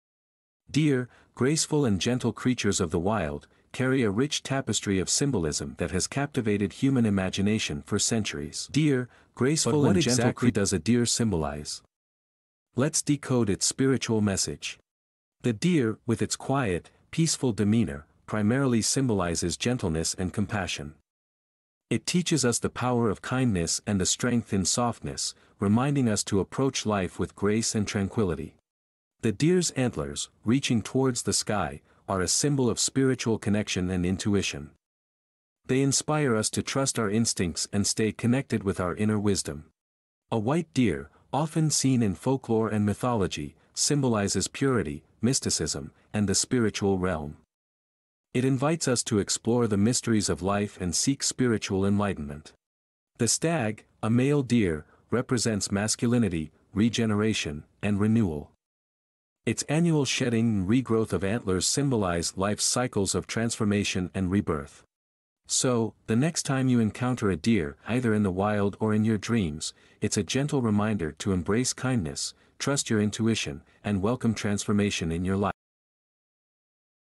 Deer Symbolism: Decoding Its Spiritual Message | FlowWithLife
Embark on a spiritual journey with us as we explore the rich symbolism of deer in this video. We'll decode the spiritual messages that deer carry, from gentleness and compassion to spiritual connection and renewal. Discover the profound meanings behind the deer's antlers, the white deer, and the stag.